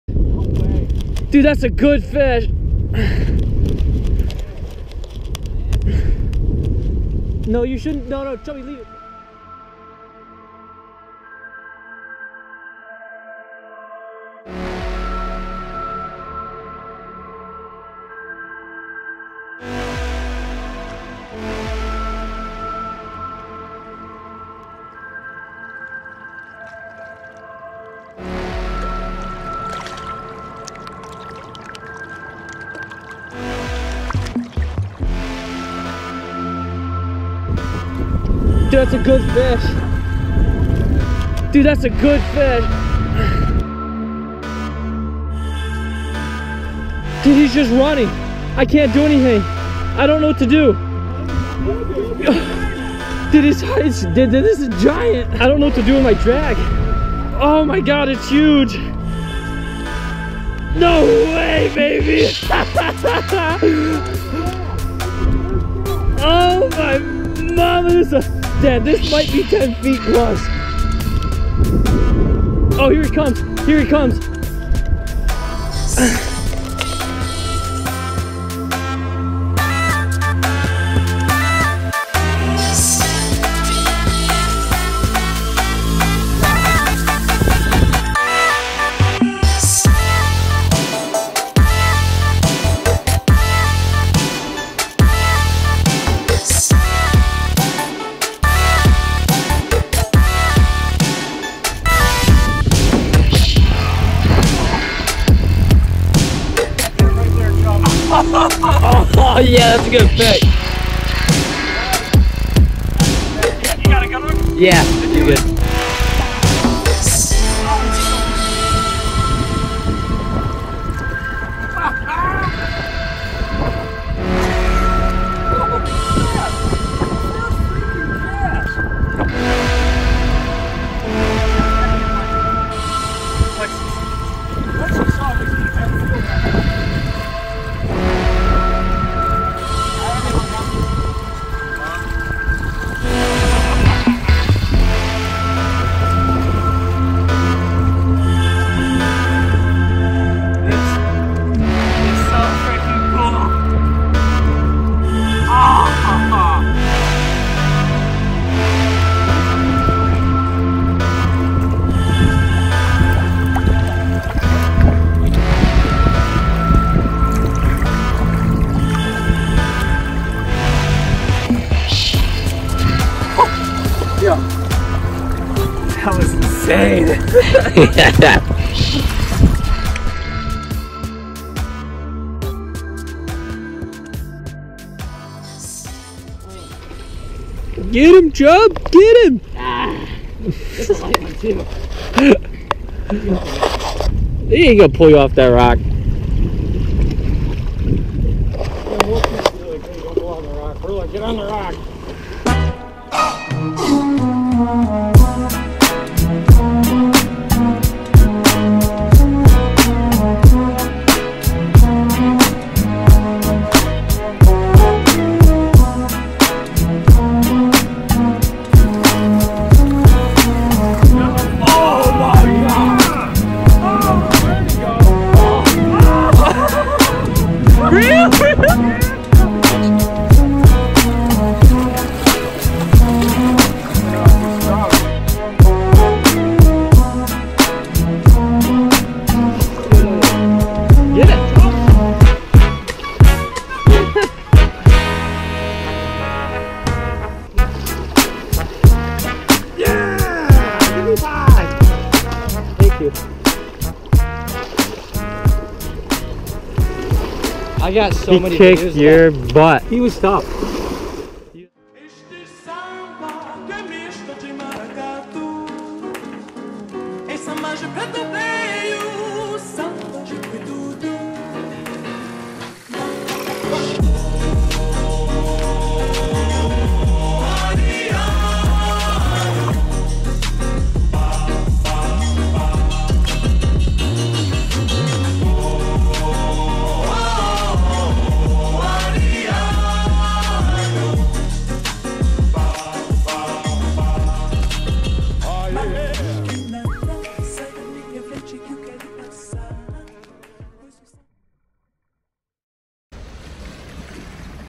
First EVER White Sturgeon -- Pannes Tour Day 1
My very first every white sturgeon came on the first cast within 15 minutes of fishing. What an unbelievable fish and an unbelievable place to fish. Cross that off my bucket list. This was an amazing start to an amazing trip and its only going to get better so stayed tuned and always fish for the moment...
--Wanna be plug